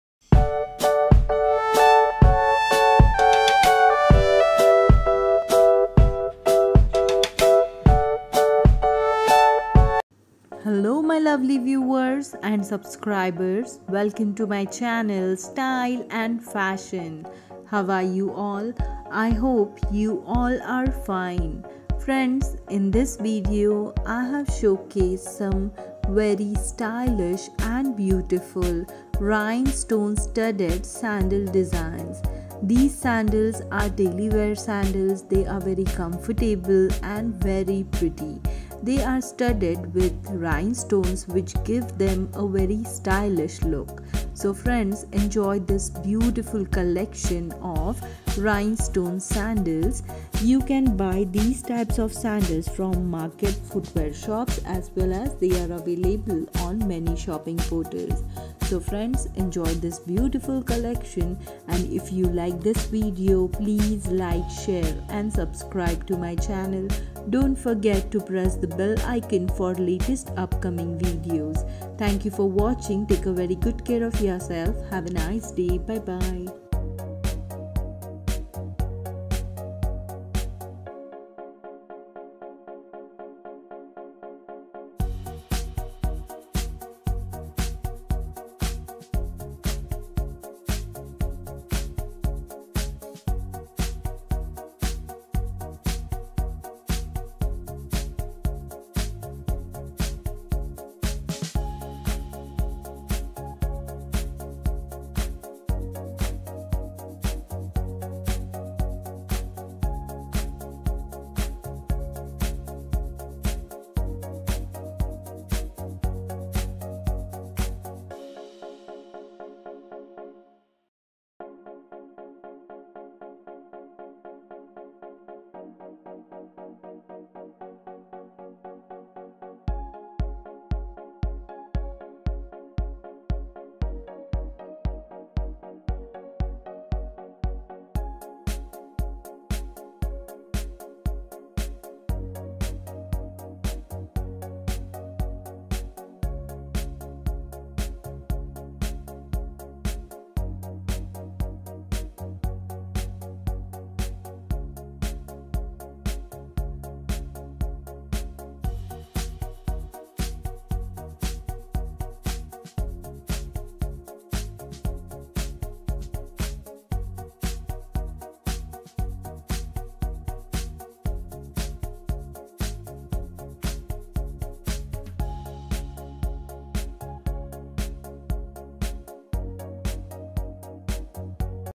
SANDAL DESIGNS || RHINESTONE STUDDED DAILY WEAR SANDAL DESIGN FOR WOMEN (ENGLISH)
Here in this video, I have shared rhinestone Studded daily Wear Sandal Designs.